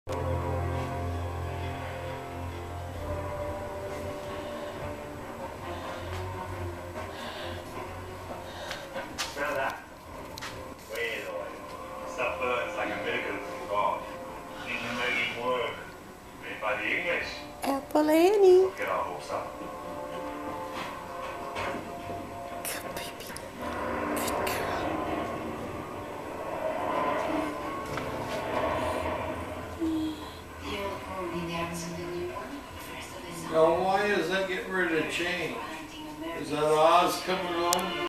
Apple Annie with Crash and St. Louie
Apple Annie at her foster home, with resident Saints Crash and St. Louie.